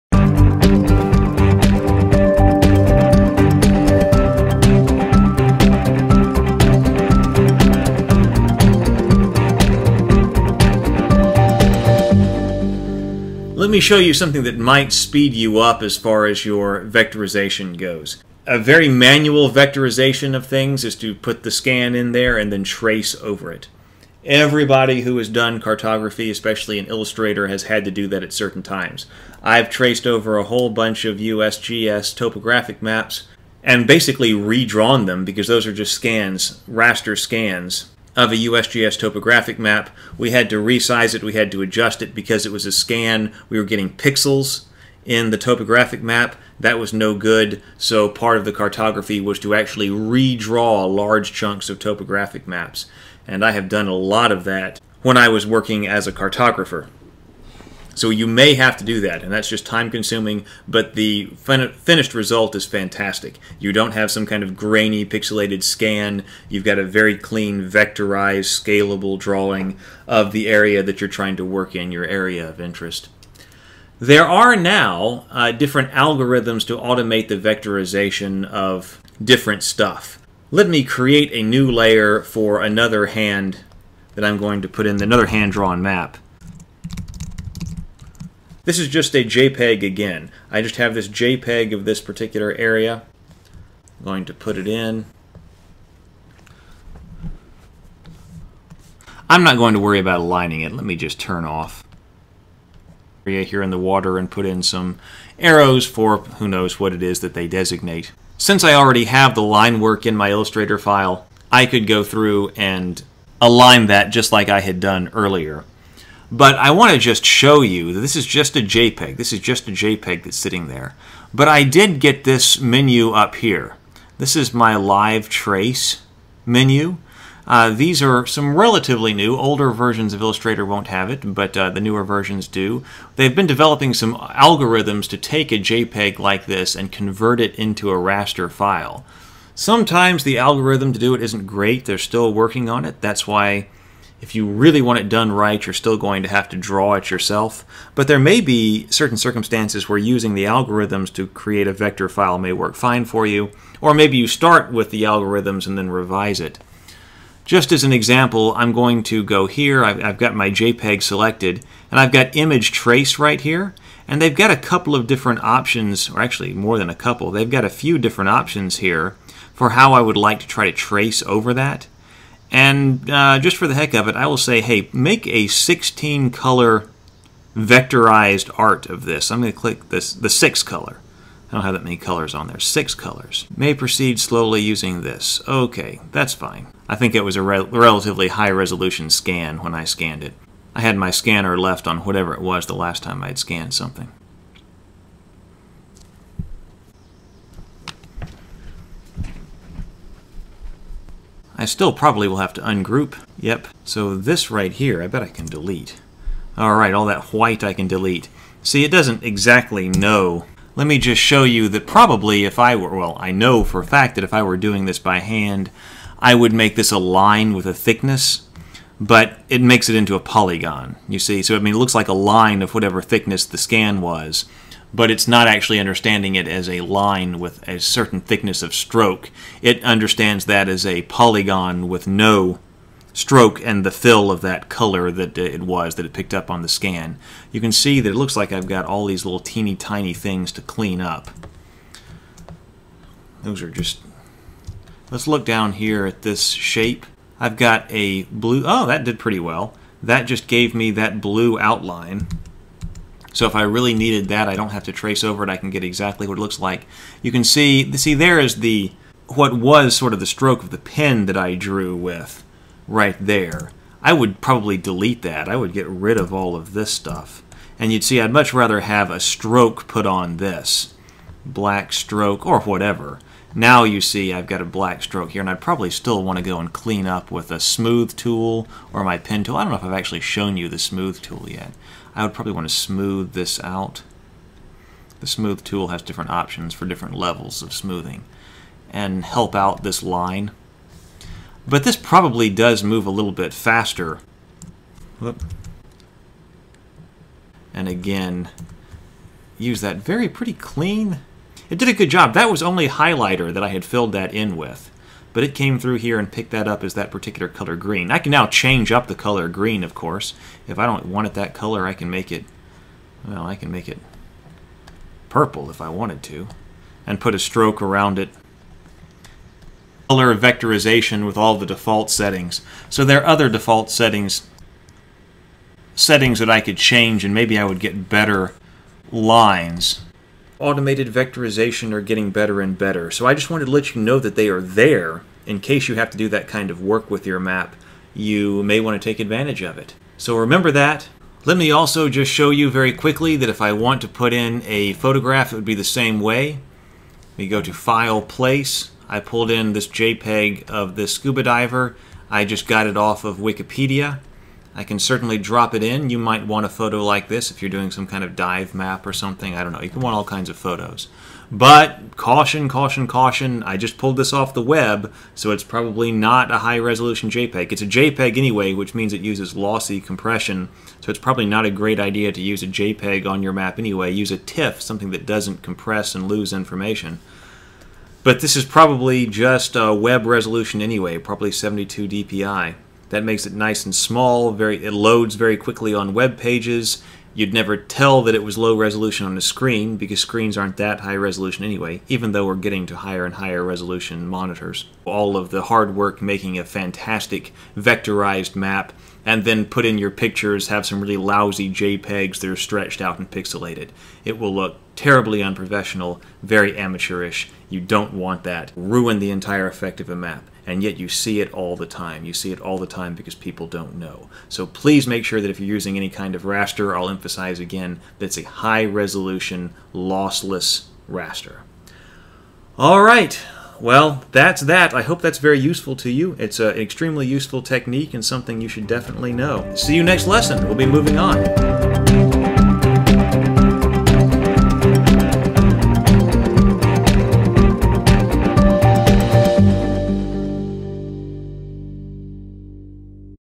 Adobe Illustrator for Cartographers 38: Automating Vectorization
In this video we look at the automating the vectorization process. It is often the case that a cartographer will receive hand-drawn maps with information that will need to be included in a vector map. Redrawing these by hand can often be tedious, but fortunantly Illustrator has tools to automate that process.